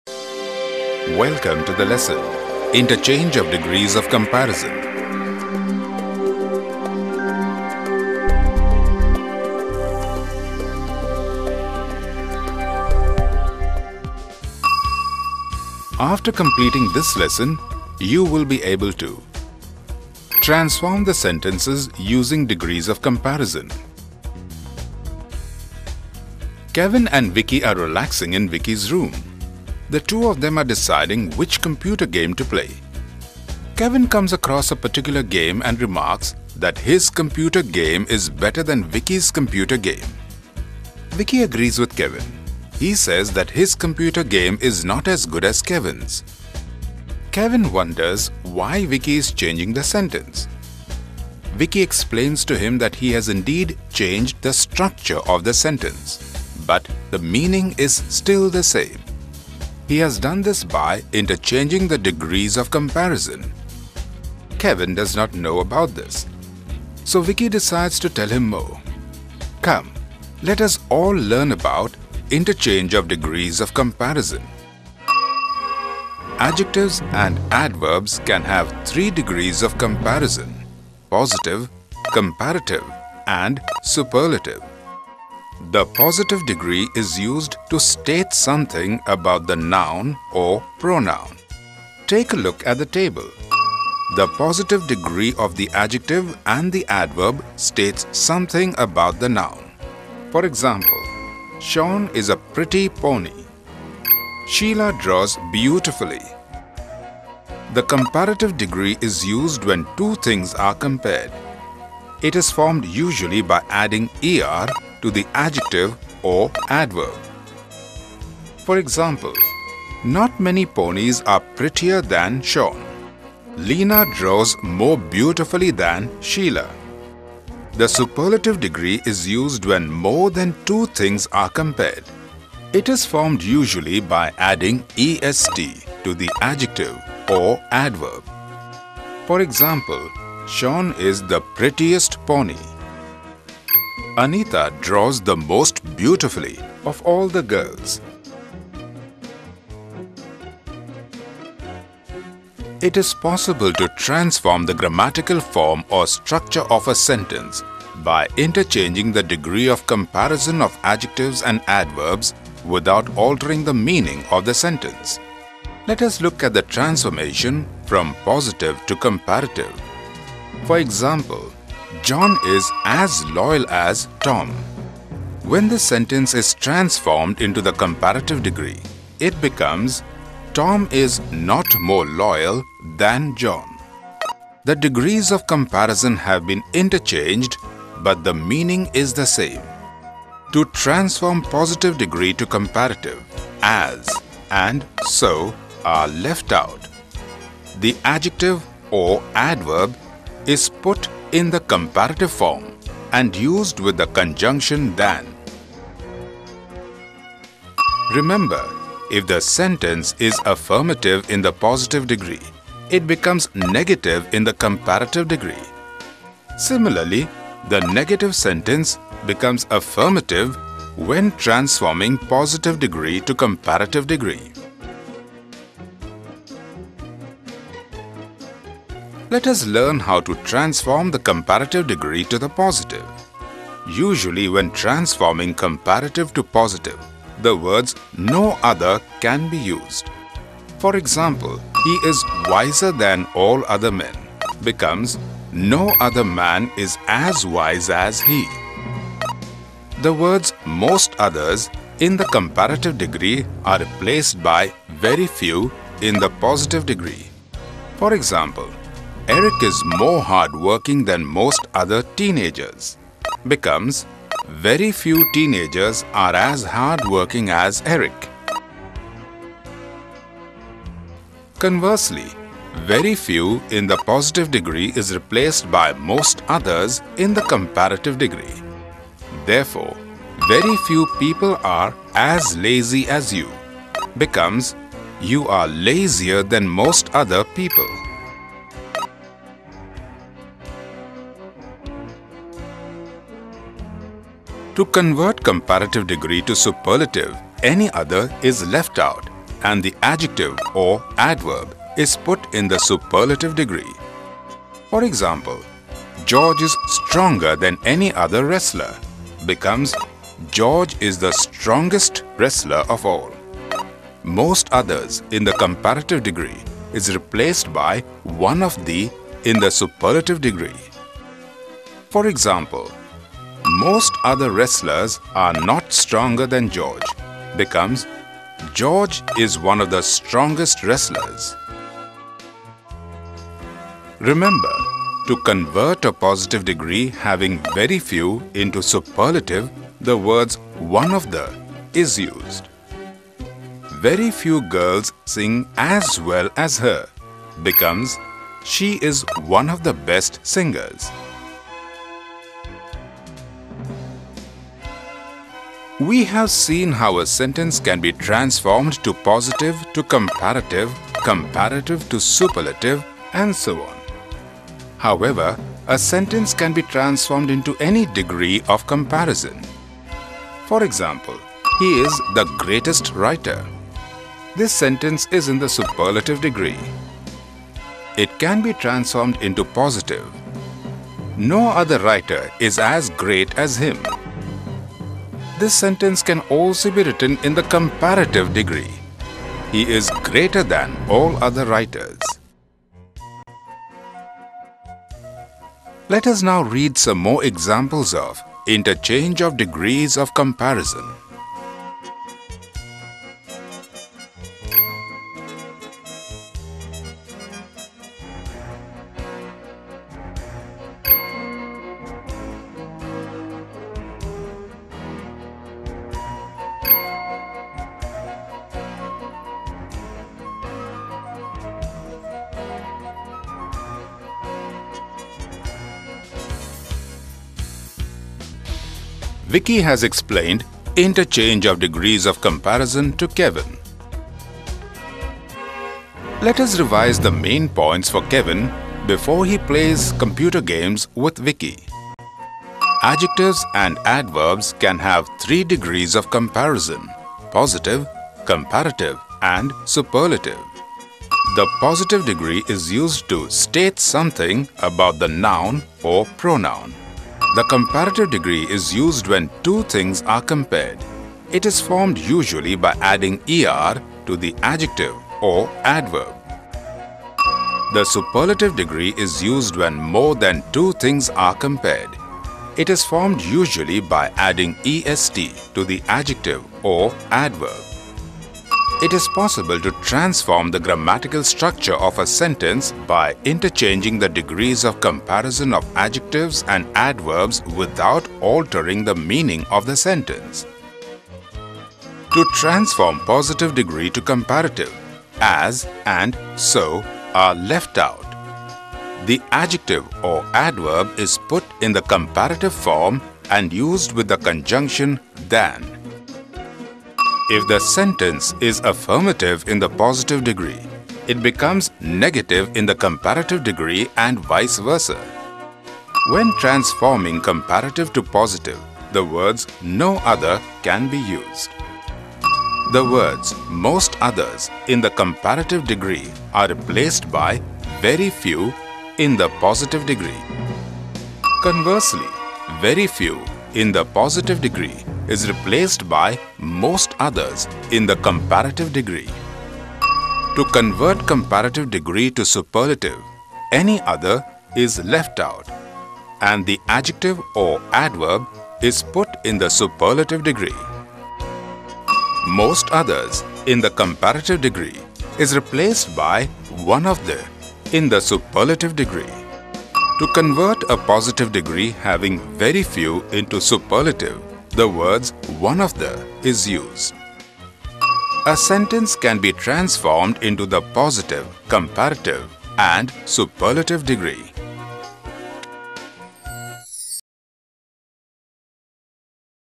Interchange of Degrees of Comparison | English Grammar | iken | ikenedu | ikenApp
This animation teaches the learner to transform the sentences using degrees of comparison.